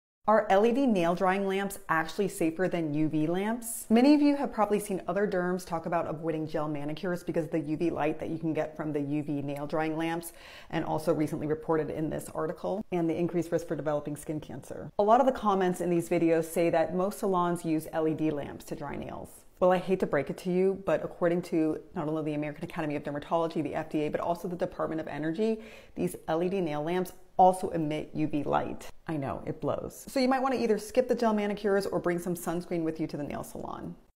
LED Lamps: Safe for gel manicures?
LED stands for "light emitting diode" and it's a method to emit light at whatever wavelength. The wavelength used in nail drying lamps fall in the UV light spectrum. The LEDs used in face masks are at a completely different (and safer) wavelength from the LEDs used in nail drying lamps. The LEDs nail lamps are in the UV wavelength spectrum. If you're getting gel manicures, think about sunscreen.